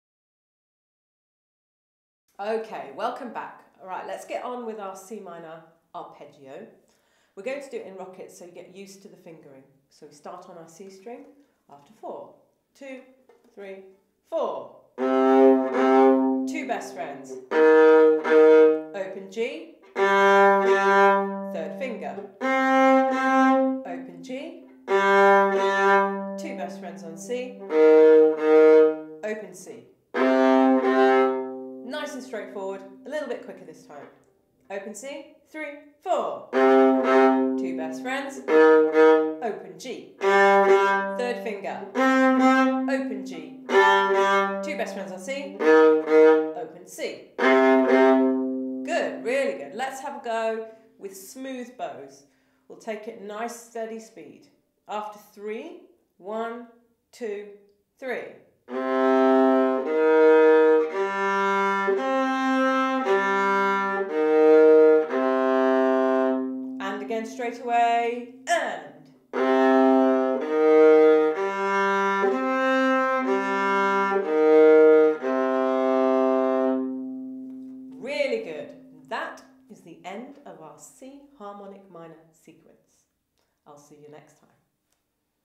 Scales Workshop - Viola Grade 2 - C Minor Arpeggio: 1 Octave
A guide through one octave of the C Minor Arpeggio for Grade 2 Viola as per the ABRSM exam syllabus, including finger positions.